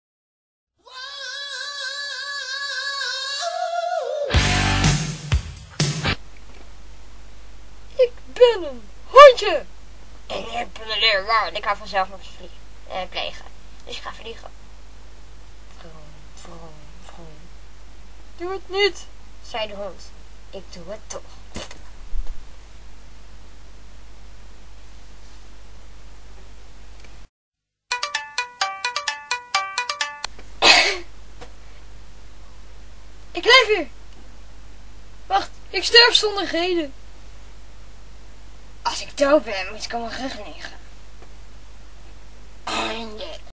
De Suicidale Leeuwenkoning(must see movie)
een leeuw die van zelfmoord houdt
eigenlijk een soort terrorist